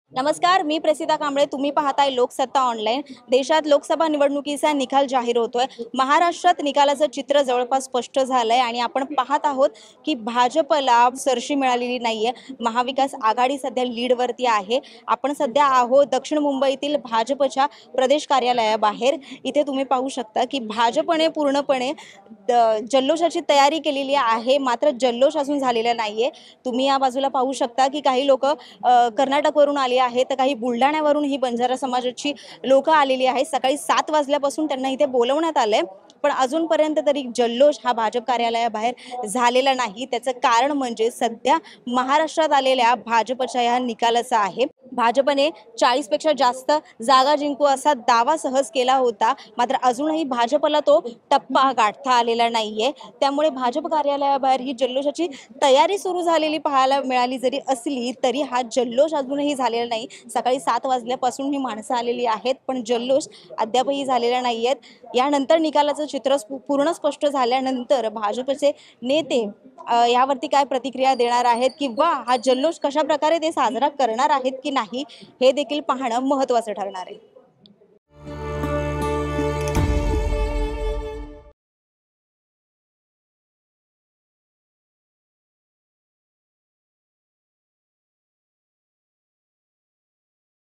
BJP Maharashtra Result: महाराष्ट्रात भाजपाची पिछेहाट, प्रदेश कार्यालयाबाहेरील चित्र काय?
महाराष्ट्रात ४० पेक्षा जास्त जागा मिळतील असा दावा भाजपाकडून करण्यात आला होता. मात्र सध्या महाराष्ट्रातील चित्र पाहता भाजपापेक्षा महाविकास आघाडीने सरशी घेतल्याचं चित्र आहे. मोठा विजयाचा दावा करण्याऱ्या भाजपाच्या मुंबईतील प्रदेश कार्यालयाबाहेरील चित्र पाहता कार्यकर्ते जल्लोषाच्या प्रतीक्षेत असल्याचं चित्र आहे. या परिस्थितीचा घेतलेला हा आढावा.